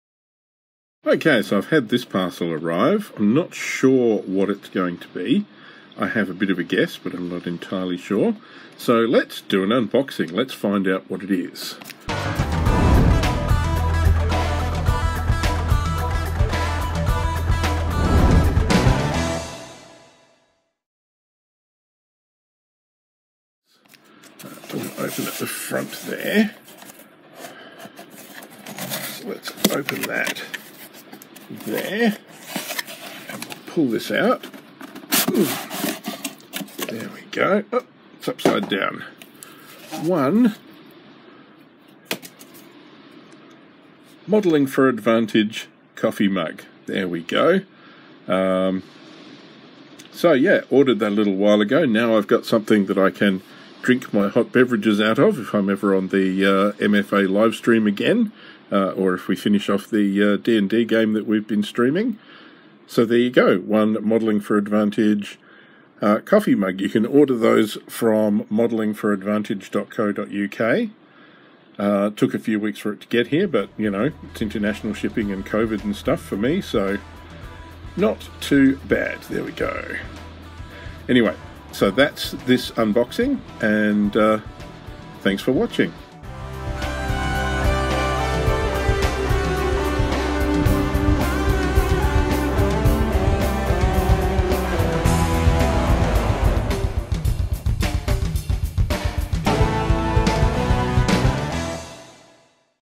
Unboxing | Mystery Unboxing - What could be in this box?
Join me for a mystery unboxing! A box arrived and I suspect I know what's in it. But you can join me as I open it and find out for sure!

Our Facebook page is Any comments and episode suggestions are welcome.

Useful Resources
Modelling For Advantage